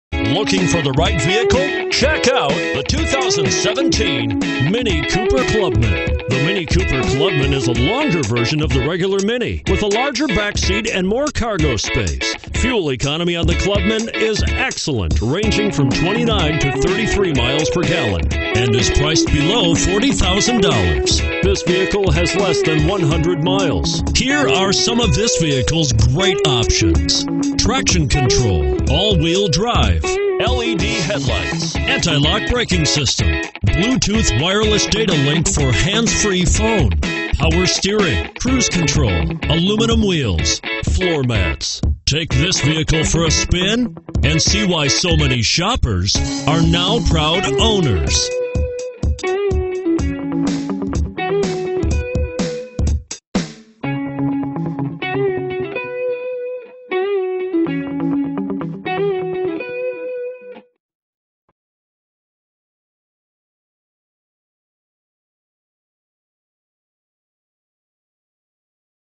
2017 MINI Clubman Kennesaw GA M8413
2017 MINI Clubman Cooper S - $39750

Looking for the right 4 door car? Today could be your lucky day. this 2017 MINI Clubman could be yours.